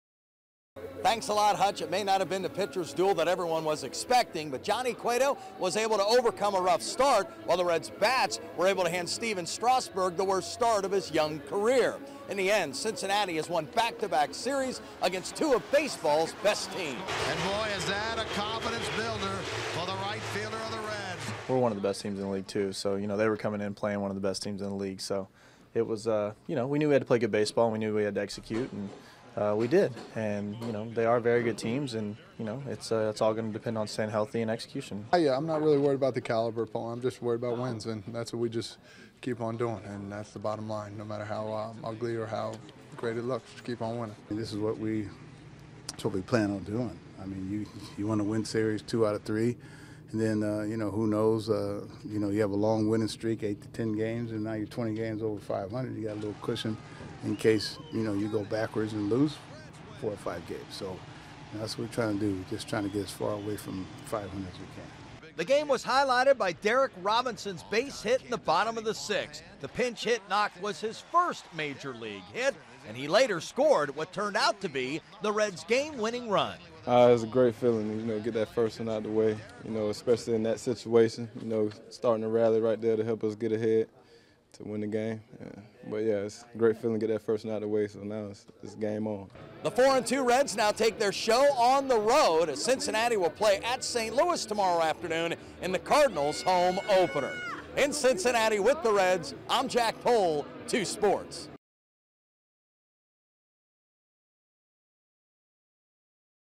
Jack Pohl reports on the Reds 4-2 start to the season
Jack Pohl reports from Great American Ball Park after the Reds won 4 of 6 to open the 2013 regular season  The Reds took 2 out of 3 from the L.A. Angels, then 2 out of 3 from the Washington Nationals.  (April 2013).